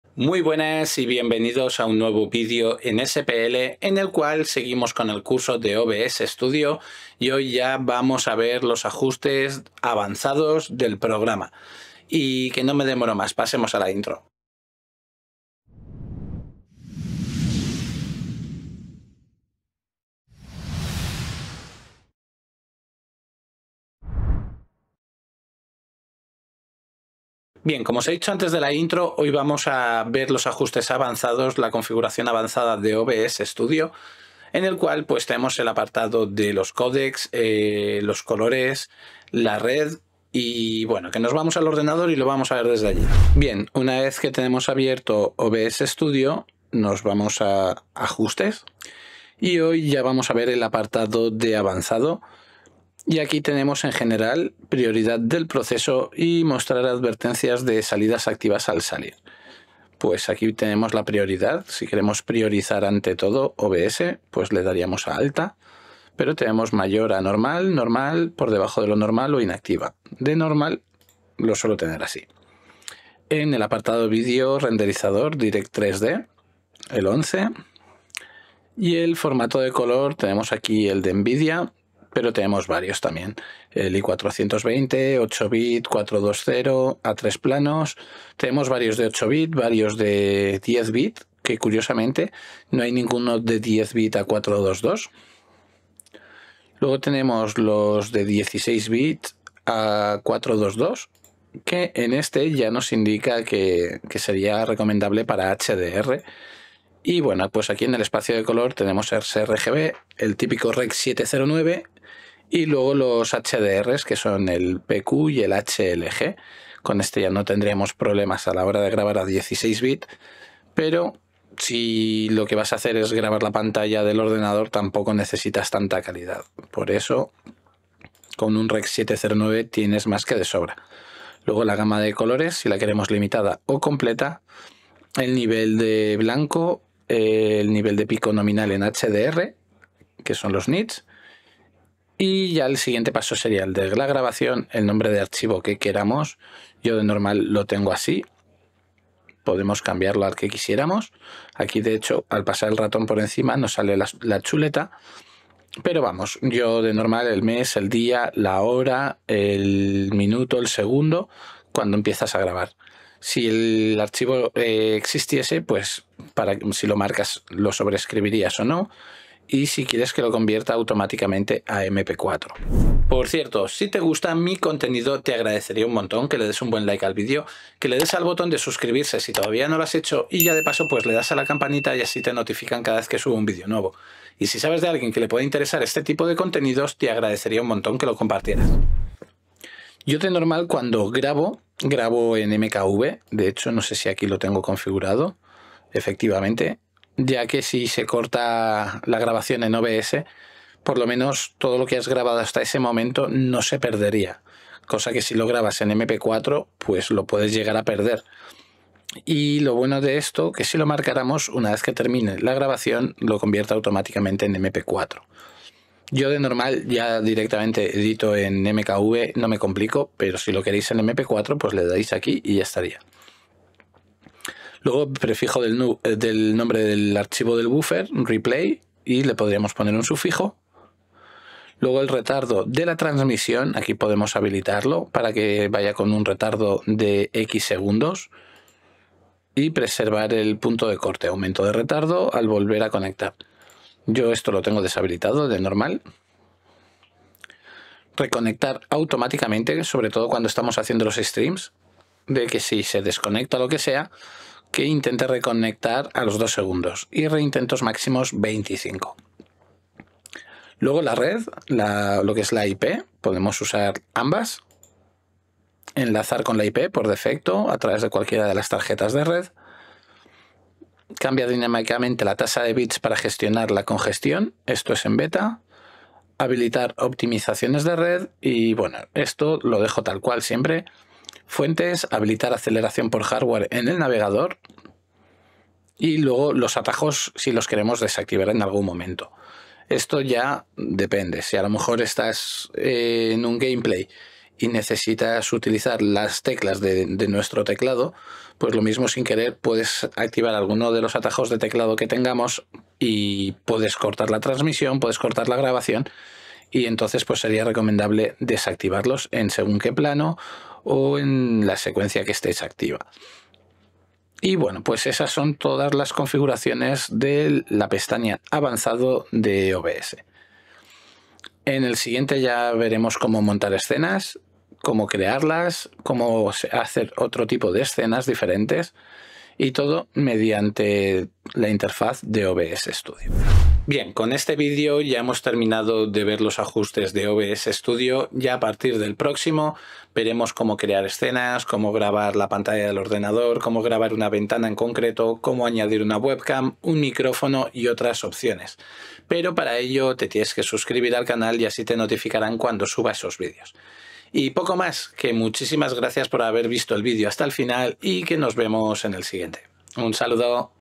DOMINA OBS STUDIO: AJUSTES AVANZADOS 🚀 | ESEPE ELE
¡Hola a todos! 🎥 En este nuevo episodio de **Esepe Ele**, continuamos explorando el fascinante mundo de OBS Studio. Hoy, nos sumergiremos en los ajustes avanzados del programa, desentrañando los secretos que harán que tu experiencia de grabación sea aún mejor. Pero sin más preámbulos, ¡vamos a sumergirnos en la intro!

Durante este recorrido por los ajustes avanzados de OBS Studio, exploraremos áreas cruciales como codecs, configuración de colores y ajustes de red. Desde la prioridad del proceso hasta las opciones de grabación, cubriremos cada detalle para optimizar tu experiencia de grabación y transmisión.

🌈 En el apartado de video, exploraremos el renderizador Direct 3D y los formatos de color, desde los clásicos como i420 hasta los recomendados para HDR en 16 bits a 422. Además, analizaremos espacios de color como RSRGB, REC709, PQ y HLG, asegurándonos de obtener la calidad que necesitamos.

En la sección de grabación, discutiremos la configuración del nombre de archivo, formatos preferidos como MKV o MP4, y opciones para preservar tus grabaciones en caso de cortes inesperados. ¡No te pierdas los trucos para convertir automáticamente tus archivos al formato que prefieras!

Para los entusiastas de la transmisión en vivo, exploraremos las opciones de red, desde la gestión de la tasa de bits hasta las optimizaciones en beta. Y, por supuesto, daremos un vistazo a las configuraciones de fuentes y atajos, con consejos sobre cuándo desactivarlos para evitar interrupciones no deseadas.

En el próximo episodio, exploraremos la creación de escenas y otras funcionalidades emocionantes de OBS Studio. ¡No te lo pierdas! Muchísimas gracias por acompañarnos hasta el final. ¡Nos vemos en el siguiente video! 👋